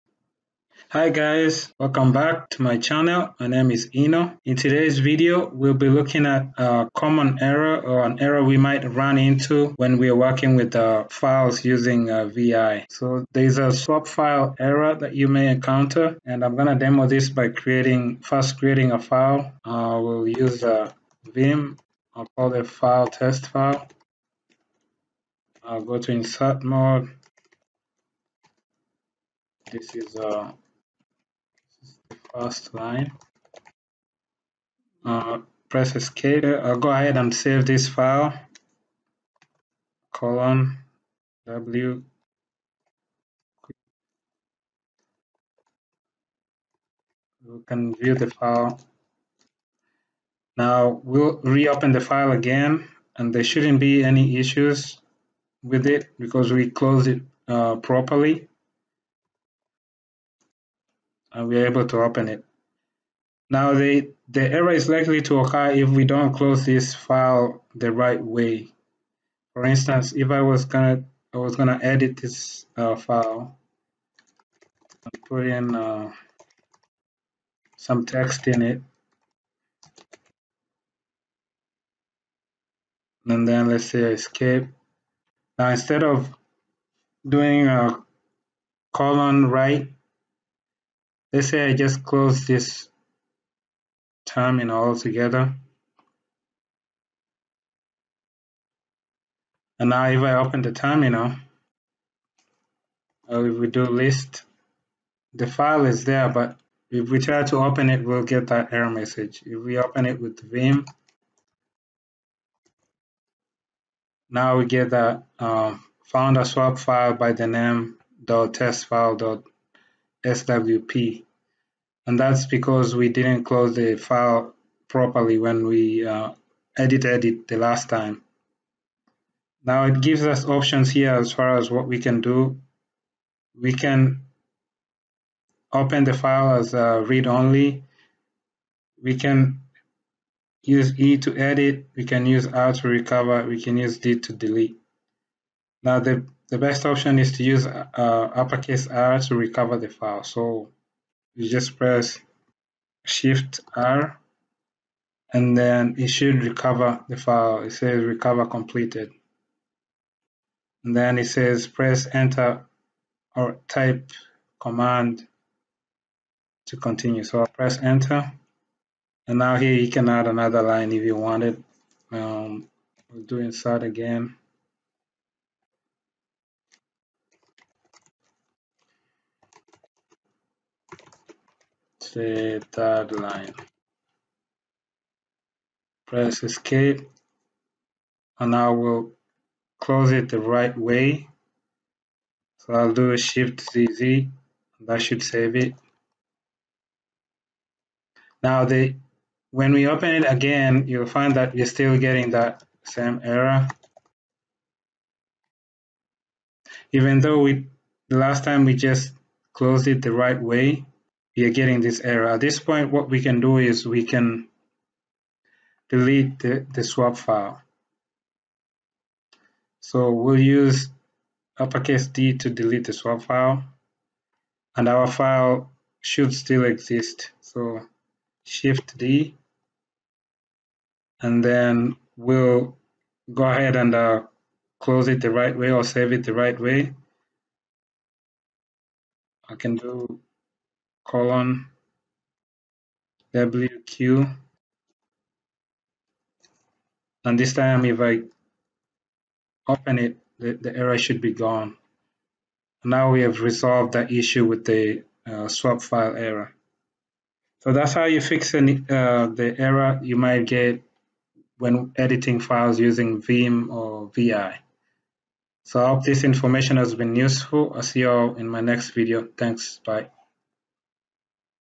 How To Fix Swap File Error When Using Vim or Vi on Linux
How to fix swap file error when opening/editing files using Vim or Vi on Linux